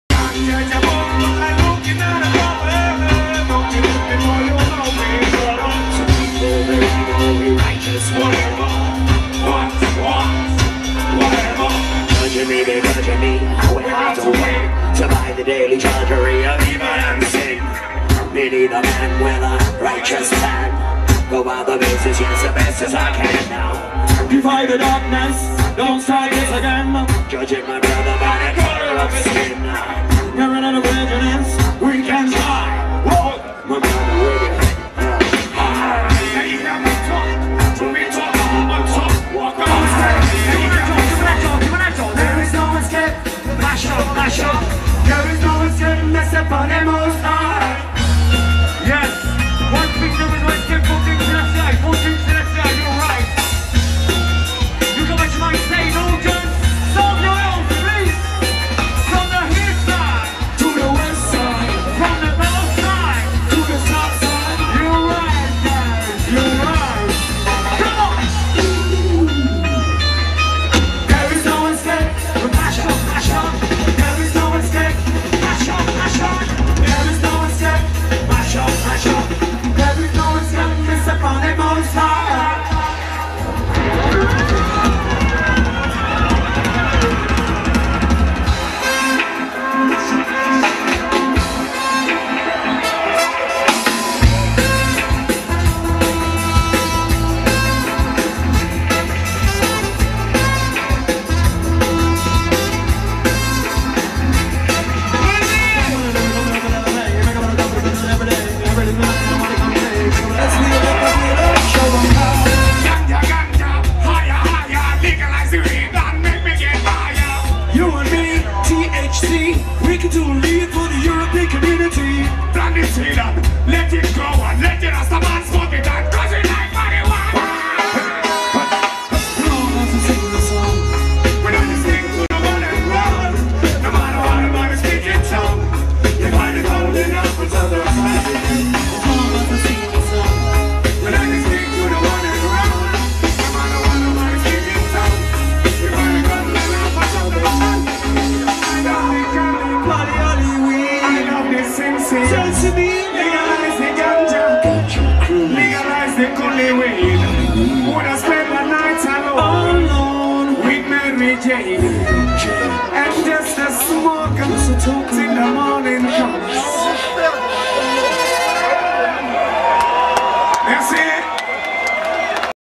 Le Peuple de L'herbe
extraits du concert du Peuple de l'Herbe au festival des Rockailles, compilés par/pour le